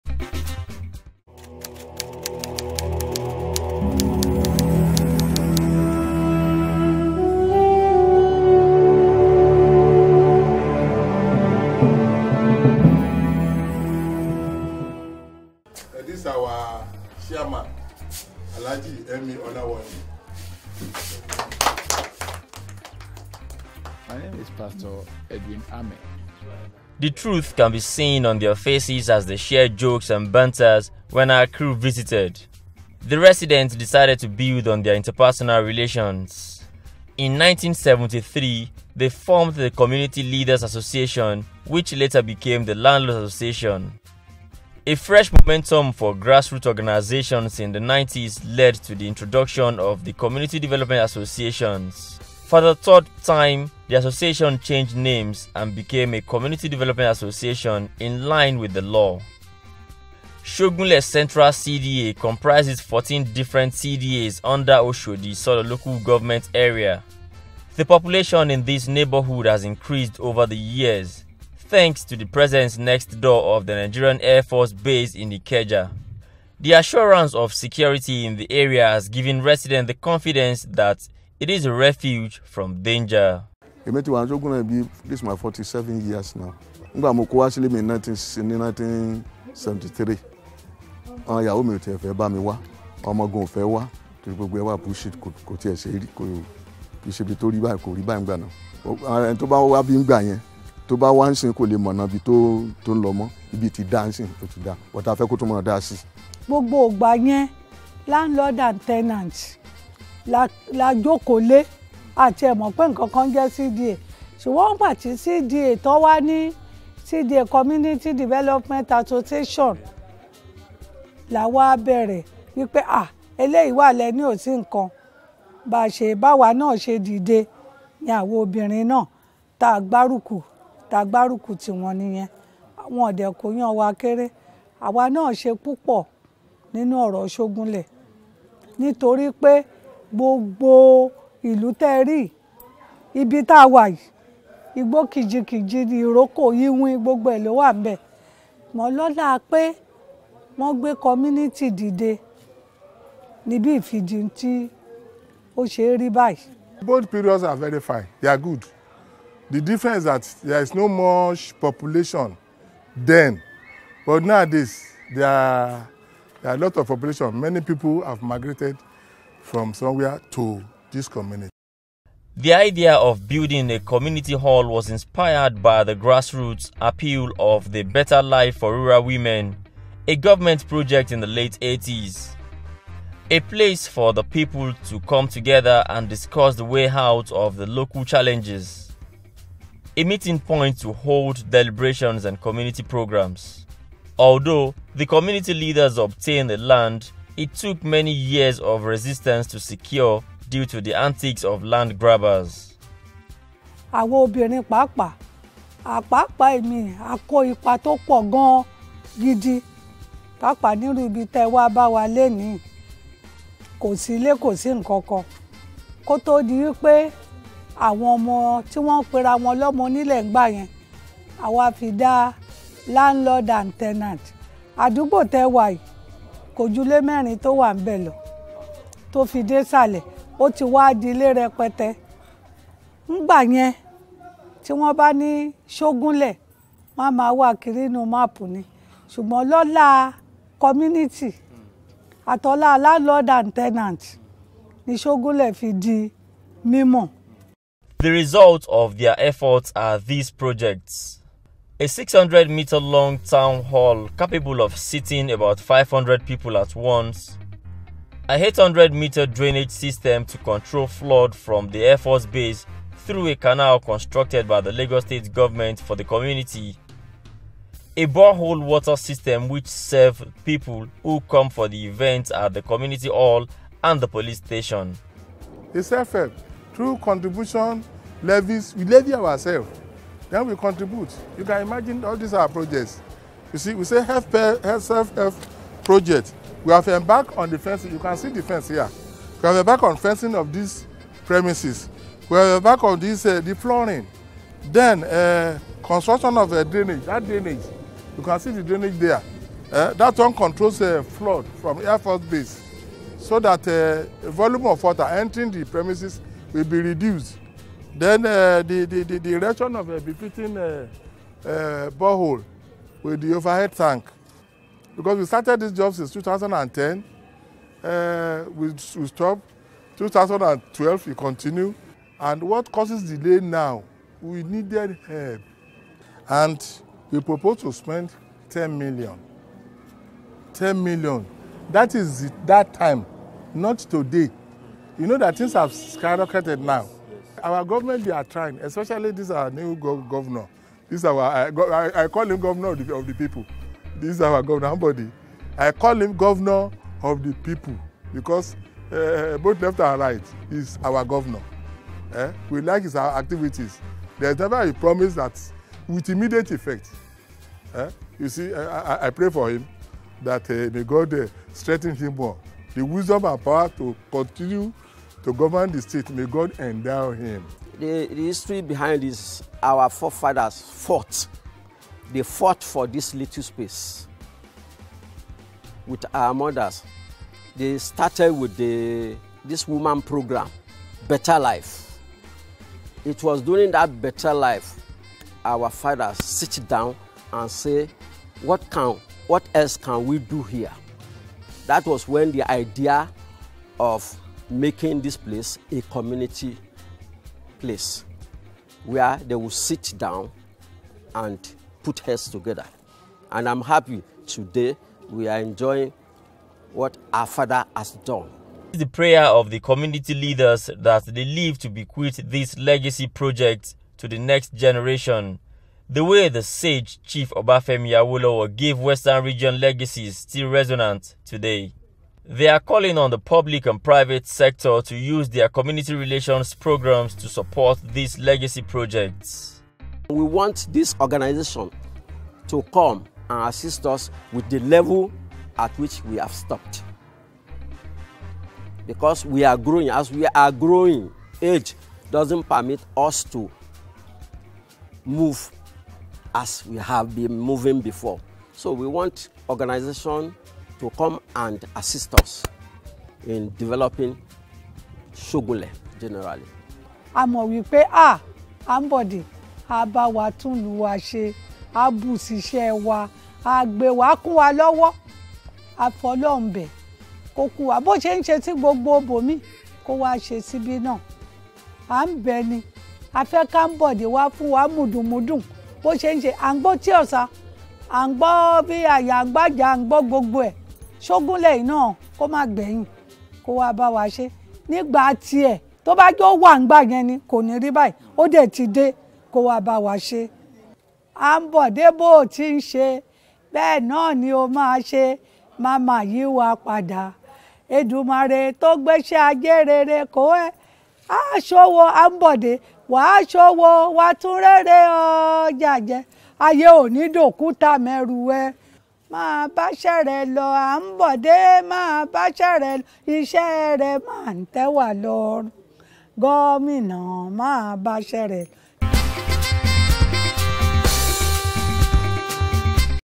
LCU18B - Report on Shogunle Central CDA - Oshodi-Isolo Local Government Area
" We want to leave a progressive legacy in our community"  - Shogunle Central Community Development Associations , Oshodi-Isolo Local Government Area